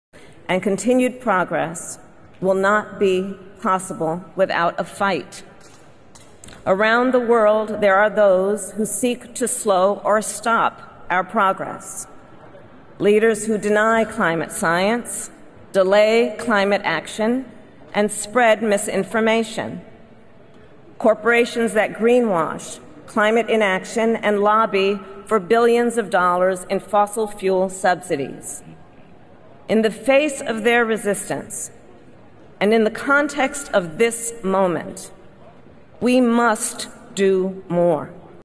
Cop28, Kamala Harris: fare di più contro la "resistenza" climatica
Vicepresidente Usa attacca negazionisti e "green washing" delle aziende